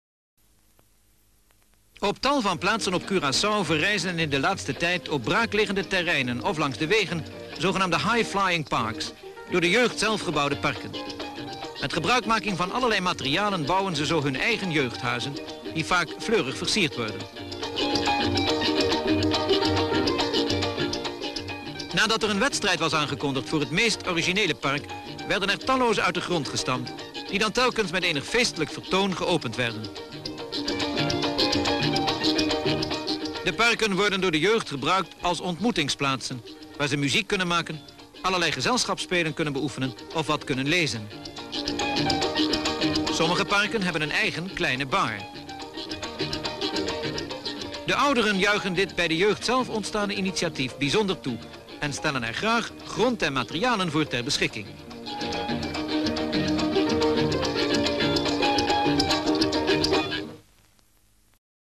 Youth builds '' High flying parks '' (Weeknummer 73-04) 🔴
Description (more): On braakliggenden areas along the roads in Curaçao builds the youth. high flying parks, youth houses built from a variety of cheerful decoration materials. After a competition for the most original park is proclaimed, there appear even more of this type of construction work. The young people meet and make music.